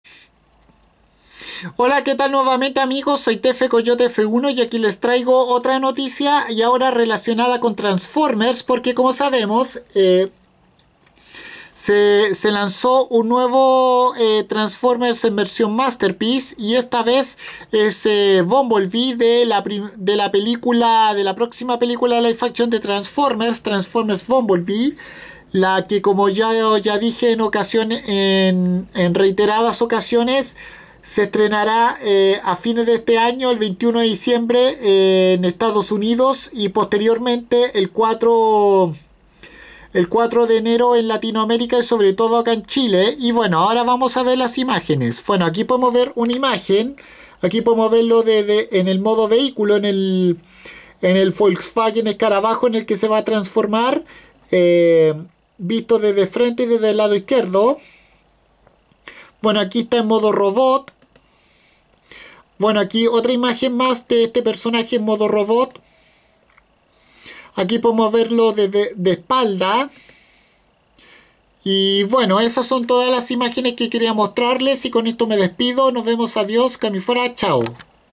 Primeras imágenes de Transformers Masterpiece Movie Bumblebee VW Beetle
Esta basada en el Bumblebee de la próxima película live action de Transformers Bumblebee The Movie que se estrenará el 21 de Diciembre en Estados Unidos y el 4 de Enero acá en Latinoamérica especialmente acá en Chile